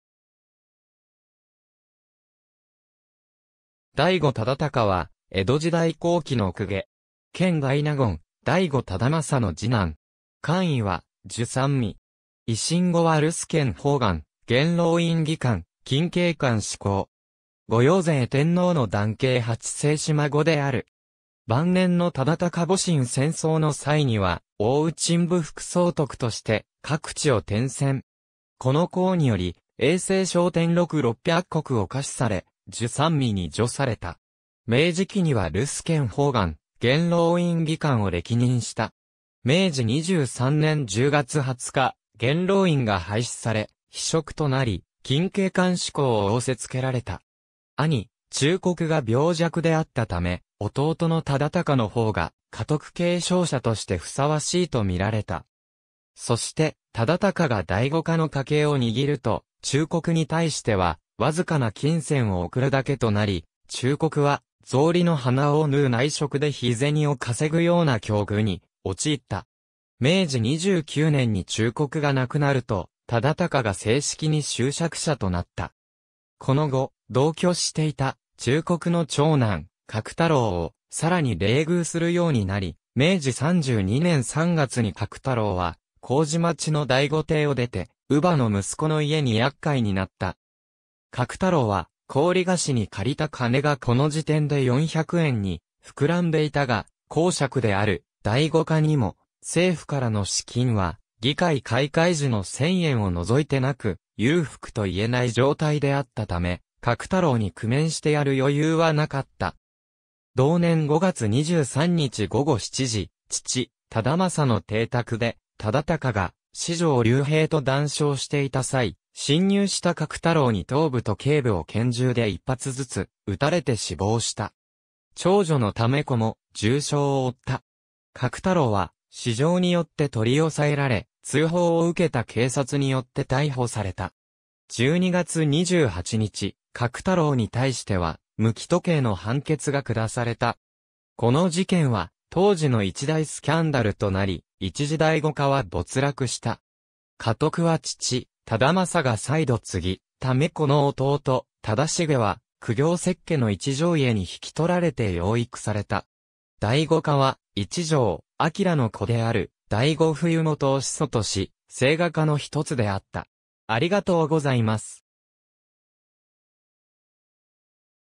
醍醐忠敬
醍醐 忠敬（だいご ただゆき）は、江戸時代後期の公家。
権大納言・醍醐忠順の次男。
官位は従三位。
維新後は留守権判官、元老院議官、錦鶏間祗候。
後陽成天皇の男系八世子孫である。
晩年の忠敬 戊辰戦争の際には奥羽鎮撫副総督として各地を転戦。
この功により永世賞典禄六百石を下賜され従三位に叙された。
明治期には留守権判官、元老院議官を歴任した。
明治23年（1890年）10月20日、元老院が廃止され非職となり錦鶏間祗候を仰せ付けられた。
兄・忠告が病弱であったため、弟の忠敬の方が家督継承者として相応しいとみられた。
そして忠敬が醍醐家の家計を握ると、忠告に対しては僅かな金銭を送るだけとなり、忠告は草履の鼻緒を縫う内職で日銭を稼ぐような境遇に陥った。
明治29年（1896年）に忠告が亡くなると、忠敬が正式に襲爵者となった。
この後、同居していた忠告の長男・格太郎を更に冷遇するようになり、明治32年（1899年）3月に格太郎は麹町の醍醐邸を出て、乳母の息子の家に厄介になった。
格太郎は高利貸しに借りた金がこの時点で400円に膨らんでいたが、侯爵である醍醐家にも政府からの賜金は議会開会時の1000円を除いて無く、裕福と言えない状態であったため、格太郎に工面してやる余裕は無かった。
同年5月23日午後7時、父・忠順の邸宅で忠敬が四条隆平と談笑していた際、侵入した格太郎に頭部と頸部を拳銃で一発ずつ撃たれて死亡した。
長女の為子も重傷を負った。
格太郎は四条によって取り押さえられ、通報を受けた警察によって逮捕された。
12月28日、格太郎に対しては無期徒刑（無期懲役）の判決が下された。
この事件は当時の一大スキャンダルとなり、一時醍醐家は没落した。
家督は父・忠順が再度継ぎ、為子の弟・忠重は公卿摂家の一条家に引取られて養育された。
醍醐家は、一条昭良の子である醍醐冬基を始祖とし、清華家の一つであった。